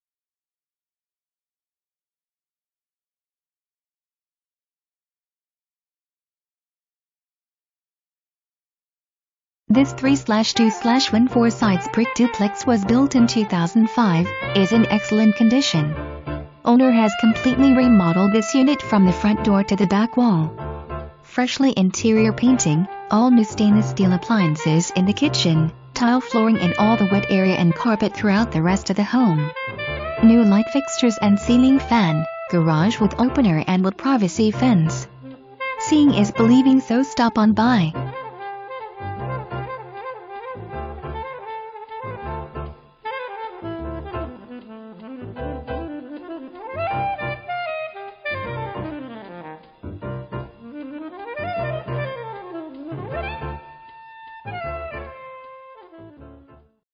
204 -B Janelle Drive Copperas Cove, Texas 76522
204- B Janelle Drive
$800, 3 bed, 2.0 bath, 1,230 SF, MLS# 326330

This 3/2/1 four sides brick duplex was built in 2005, is in excellent condition. Owner has completely remodeled this unit from the front door to the back wall. Freshly interior painting, all new stainless steel appliances in the kitchen, tile flooring in all the wet area and carpet throughout the rest of the home. New light fixtures and ceiling fan, garage with opener and wood privacy fence. Seeing is believing so stop on by

Presented By:
Robert Herrings, CENTURY 21 Premier Realtors